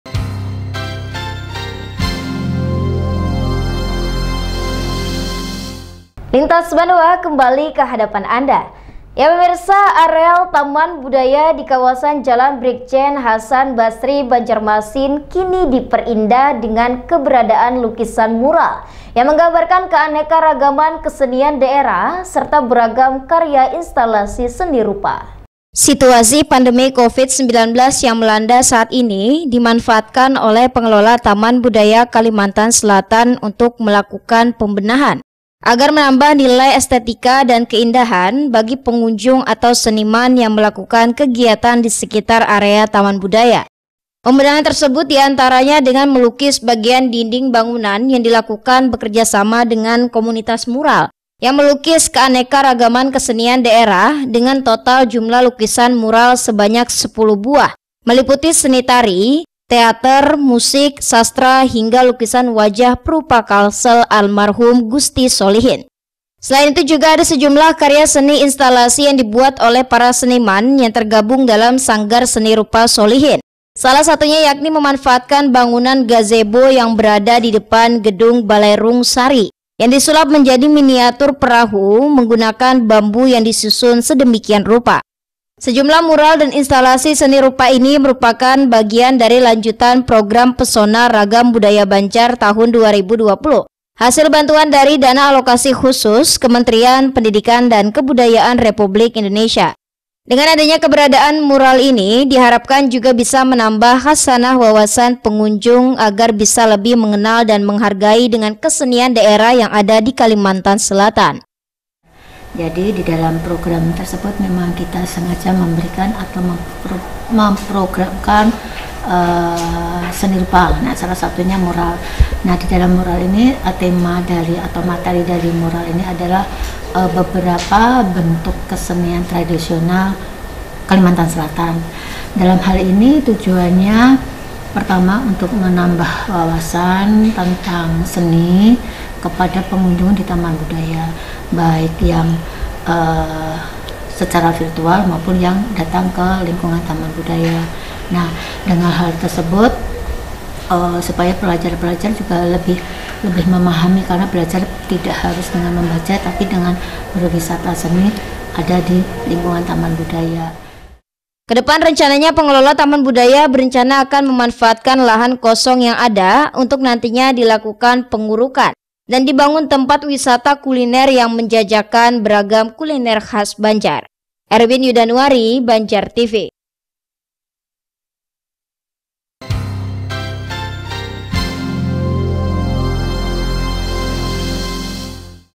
PAMERAN MURAL & INSTALASI UPTD TAMAN BUDAYA KALSEL
Areal Taman Budaya di kawasan jalan Brigjen Hasan Basri Banjarmasin, kini diperindah dengan keberadaan lukisan mural yang menggambarkan keaneka ragaman kesenian daerah serta beragam karya instalasi seni rupa.